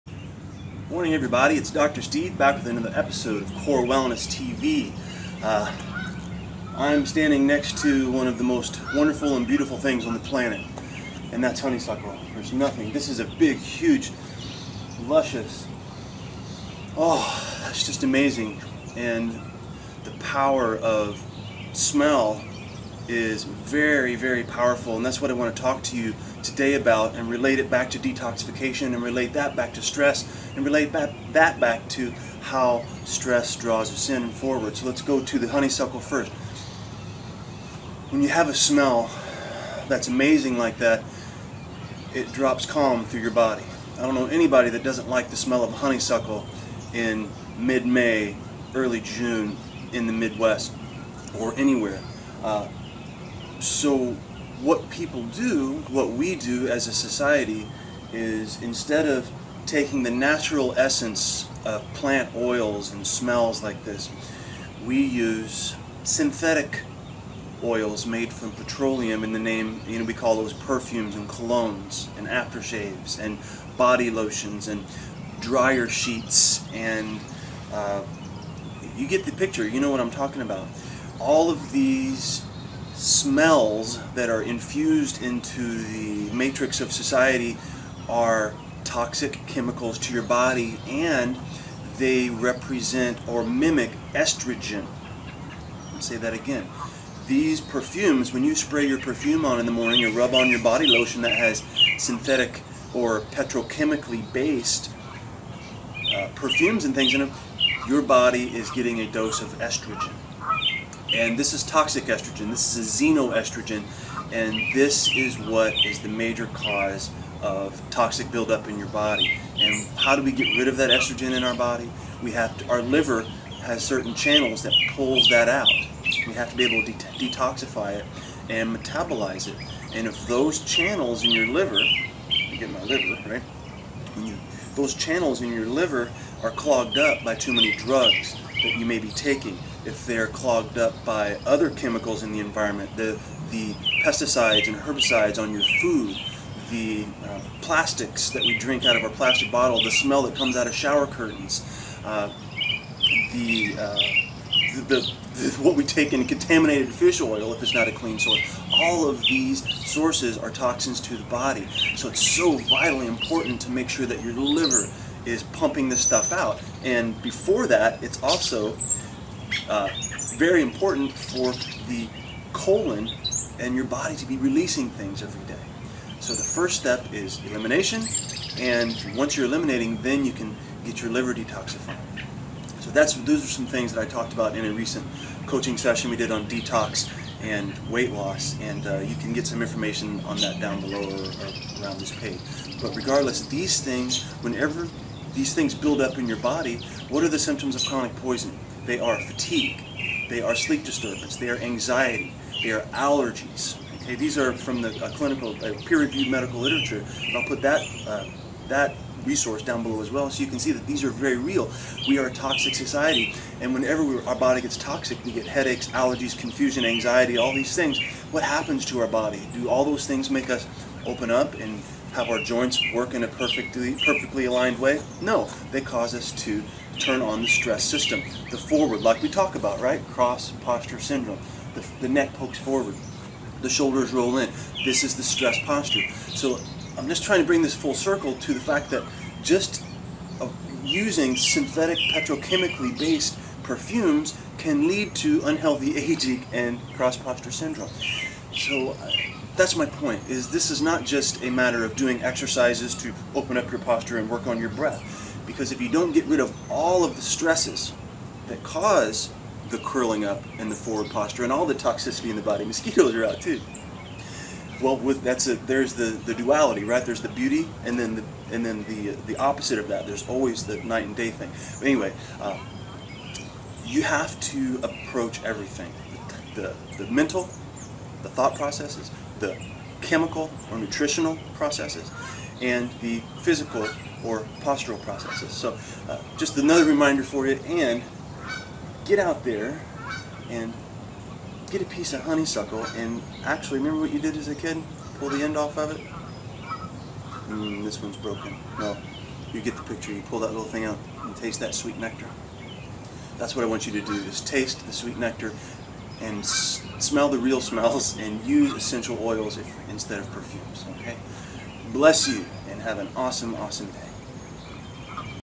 Why You Should Never Wear Perfume or Cologne
Find out why artificial scents can cause pain, anxiety, sleep disturbance, and ultimately...unhealthy aging.  The resources mentioned will be posted on the blog soon.